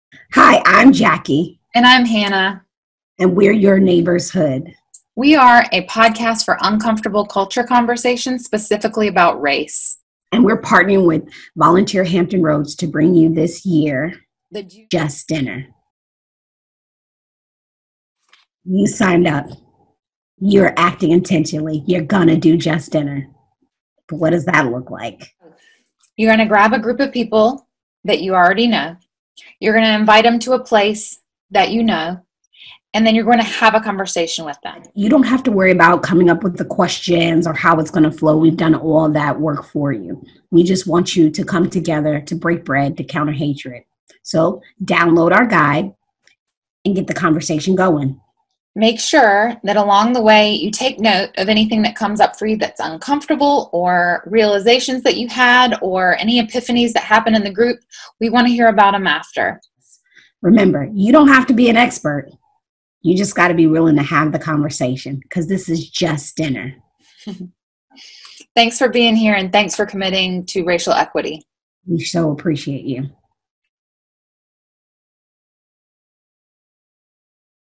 Just Dinner Intro
Breaking Bread to Counter Racism
Learn more about Just Dinner with Your Neighbor's Hood hosts Jackie and Hannah